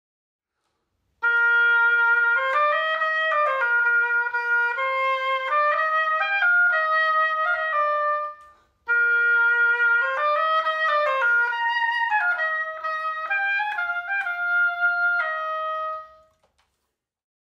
Mönnig oboe 155AM in Mopane
Beautiful Gebrüder Mönnig oboe AM155 in Mopane wood at AJ Musique in Paris 😍